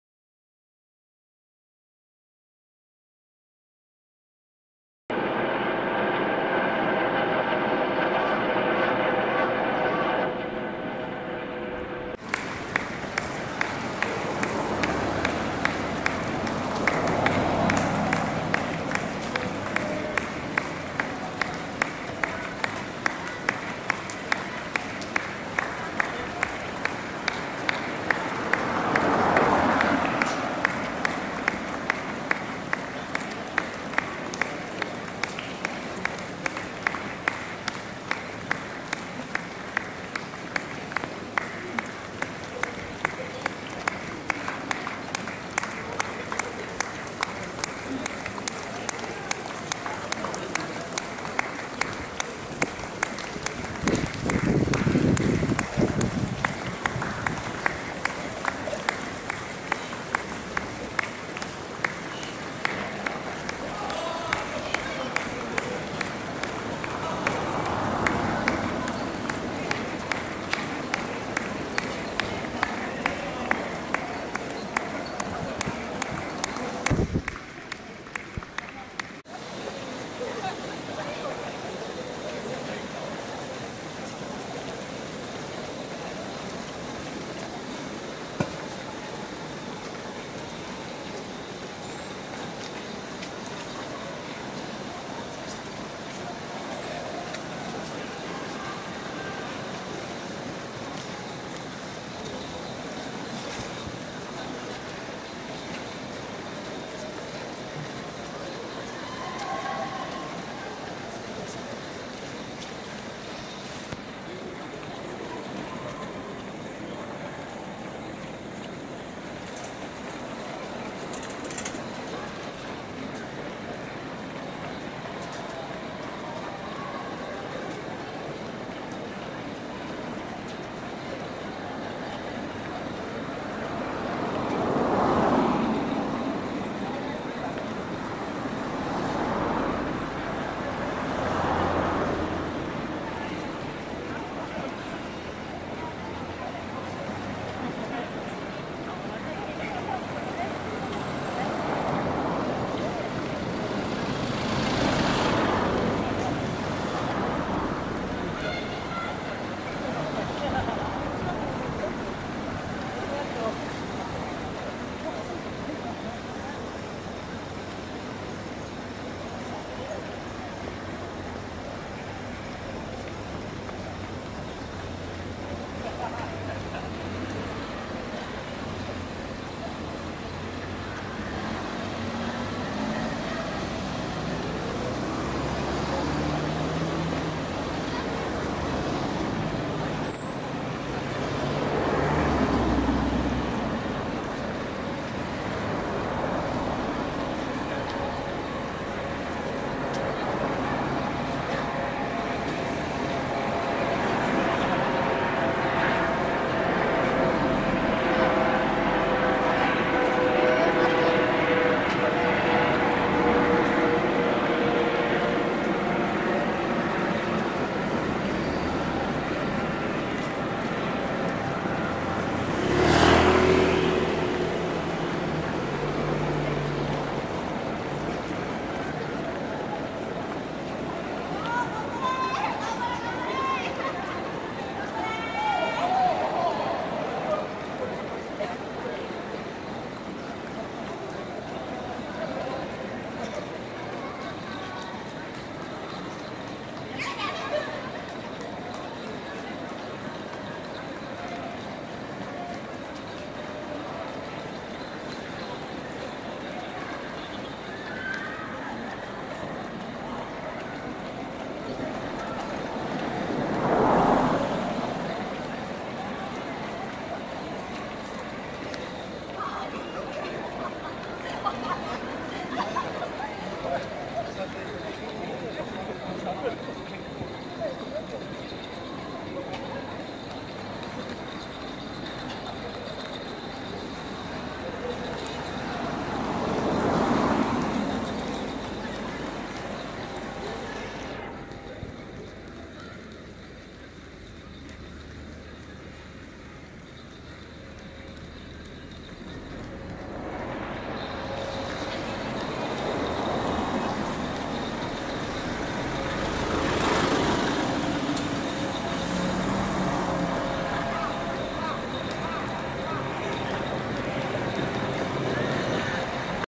FUKUOKA MARATHON 2016 - ”福岡マラソン2016”ランナーの行列にビックリ！
FUKUOKA MARATHON 2016 - 約1万3千人のランナーが福岡市から糸島市まで都市部から海沿いと変化する美しい景色を見ながら走ります。